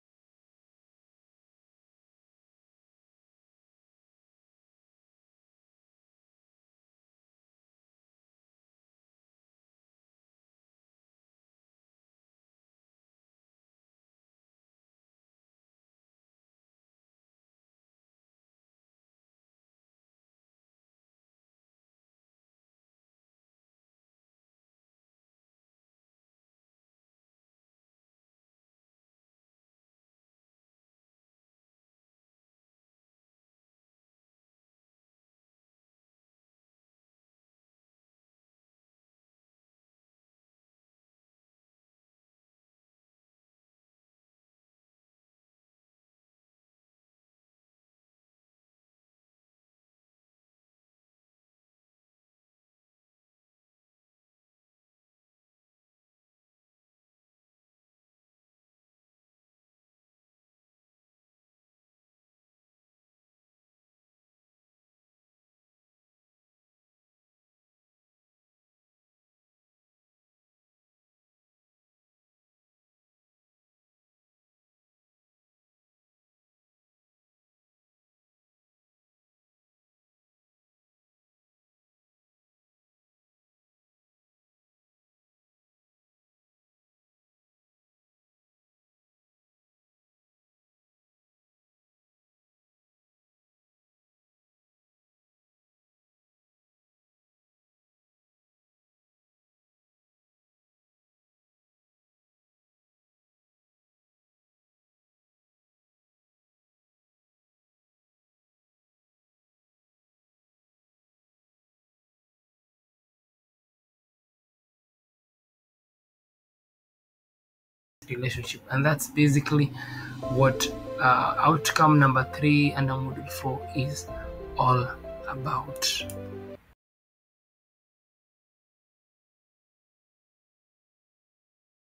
Inside Out Separation Coaching Program |Module04-Outcome03
3. STATE the grounds on which the present relationship would be ended?
The success of the new chapter of the same relationship will need an equal playing field for both parties involved in the relationship.

The inside Out Separation program is the process through which you take an emotional, mental, and spiritual retreat into the individual you are before you became a wife, or husband to ascertain your level of participation in the success of the marriage relationship.

Learn more On Inside Out Separation Program